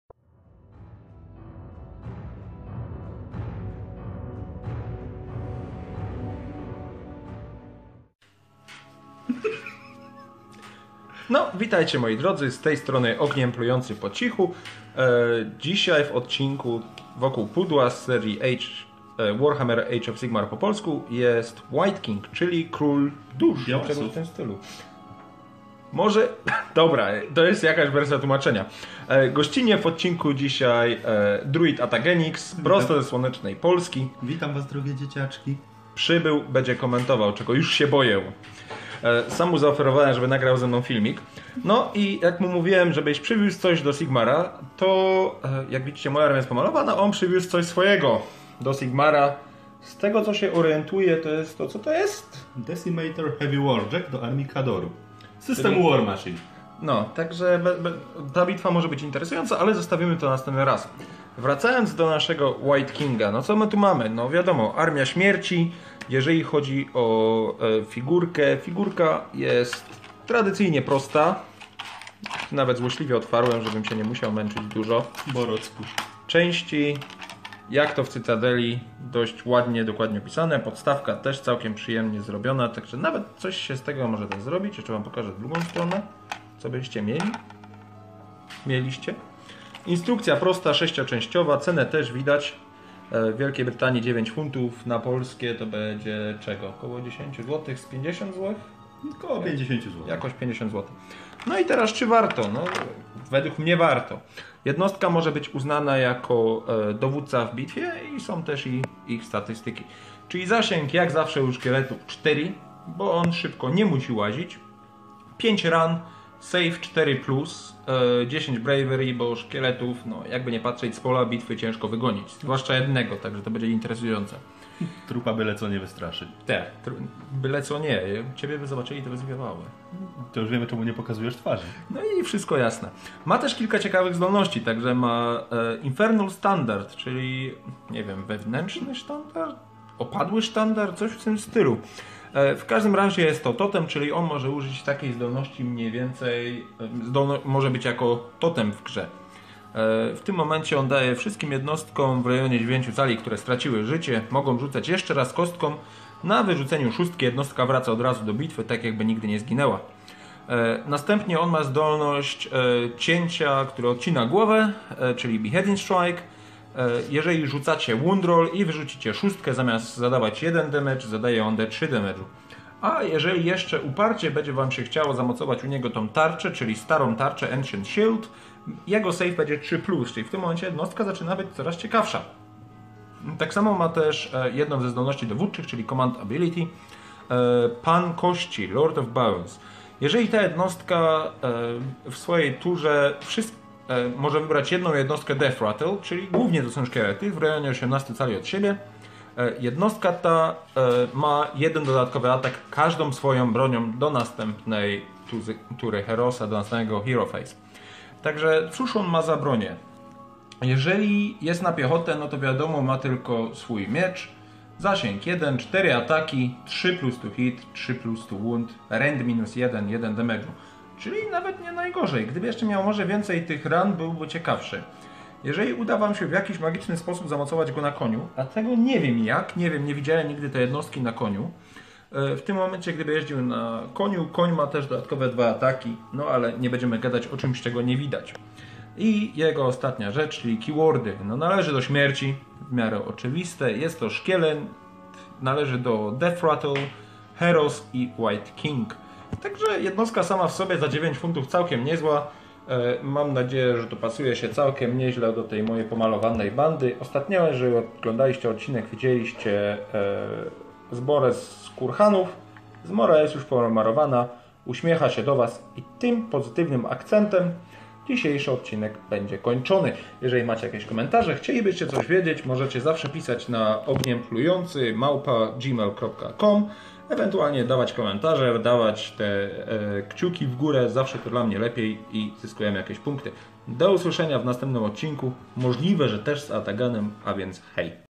Warhammer: Age of Sigmar po Polsku - Wokół pudła: Death - wight king
W tym odcinku wraz z druidem Attagenem będziemy omawiać króla upiorow., Czyli kolejna już jednostkę z wielkiej wspólnoty śmierci.  Miłego oglądania